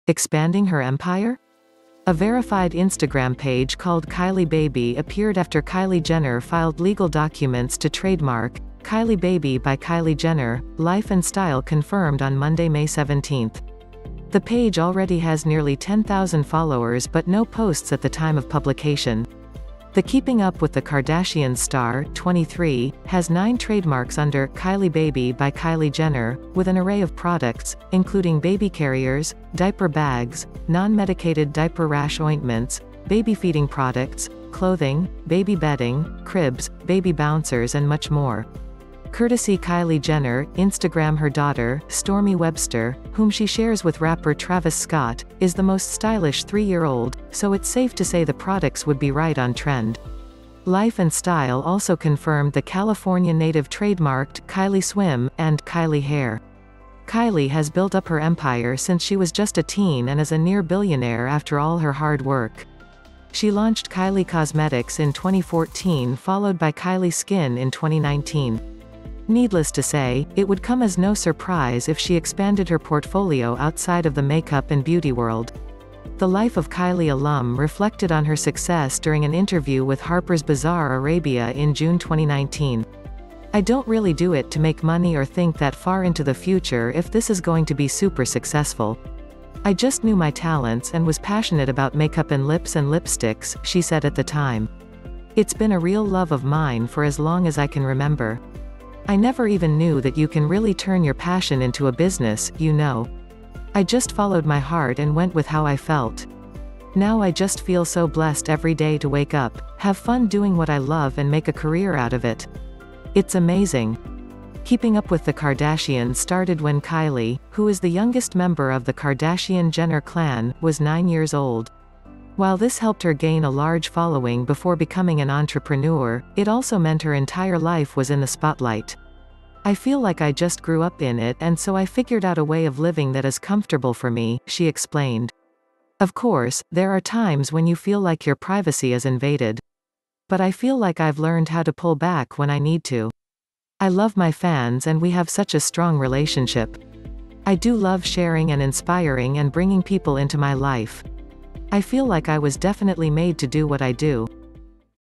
Coming Soon KylieBaby Appeared on Instagram and It's Verified
Expanding her empire? A verified Instagram page called KylieBaby appeared after Kylie Jenner filed legal documents to trademark “Kylie Baby by Kylie Jenner,” Life & Style confirmed on Monday, May 17. The page already has nearly 10,000 followers but no posts at the time of publication. The Keeping Up With the Kardashians star, 23, has nine trademarks under “Kylie Baby by Kylie Jenner” with an array of products, including baby carriers, diaper bags, non-medicated diaper rash ointments, baby feeding products, clothing, baby bedding, cribs, baby bouncers and much more. Courtesy Kylie Jenner/InstagramHer daughter, Stormi Webster, whom she shares with rapper Travis Scott, is the most stylish 3-year-old, so it’s safe to say the products would be right on trend. Life & Style also confirmed the California native trademarked “Kylie Swim” and “Kylie Hair.”Kylie has built up her empire since she was just a teen and is a near-billionaire after all her hard work. She launched Kylie Cosmetics in 2014 followed by Kylie Skin in 2019. Needless to say, it would come as no surprise if she expanded her portfolio outside of the makeup and beauty world. The Life of Kylie alum reflected on her success during an interview with Harper’s Bazaar Arabia in June 2019.“I don’t really do it to make money or think that far into the future if this is going to be super-successful. I just knew my talents and was passionate about make-up and lips and lipsticks,” she said at the time. “It’s been a real love of mine for as long as I can remember. I never even knew that you can really turn your passion into a business, you know. I just followed my heart and went with how I felt. Now I just feel so blessed every day to wake up, have fun doing what I love and make a career out of it. It’s amazing!”KUWTK started when Kylie, who is the youngest member of the Kardashian-Jenner clan, was 9 years old. While this helped her gain a large following before becoming an entrepreneur, it also meant her entire life was in the spotlight.“I feel like I just grew up in it and so I figured out a way of living that is comfortable for me,” she explained. “Of course, there are times when you feel like your privacy is invaded. But I feel like I’ve learned how to pull back when I need to. I love my fans and we have such a strong relationship. I do love sharing and inspiring and bringing people into my life. I feel like I was definitely made to do what I do.”